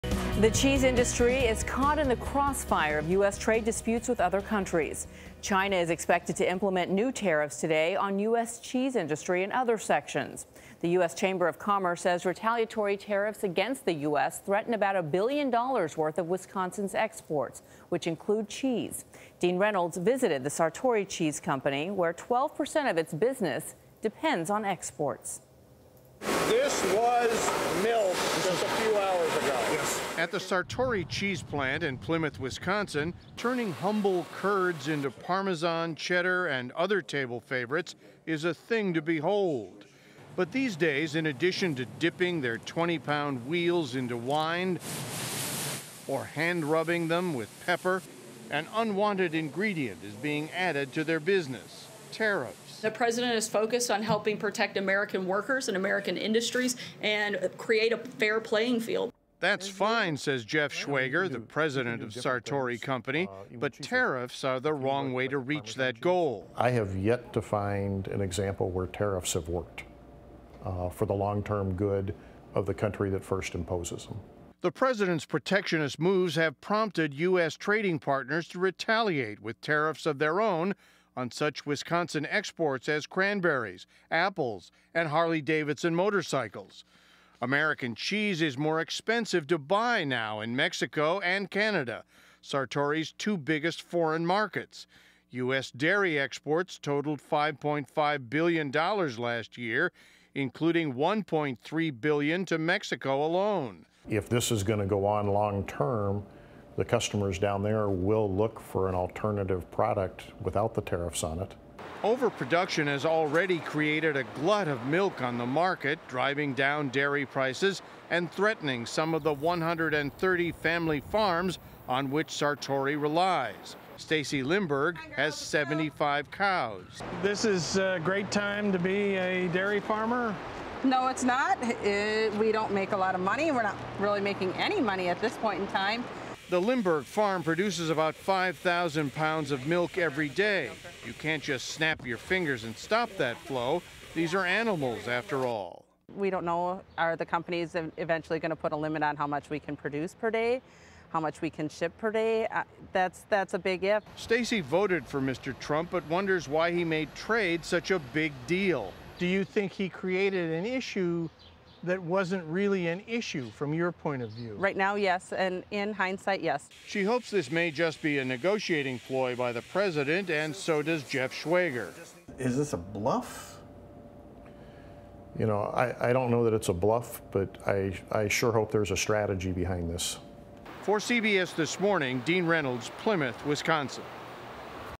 Wisconsin cheese industry braces for impact from tariffs
The cheese industry is caught in the crossfire of U.S. trade disputes with other countries. As the U.S. imposes tariffs on $34 billion worth of Chinese goods, China is expected to implement retaliatory tariffs on the U.S. cheese industry and other sectors. Dean Reynolds visits Sartori Cheese company in Wisconsin, where 12 percent of its business depends on exports.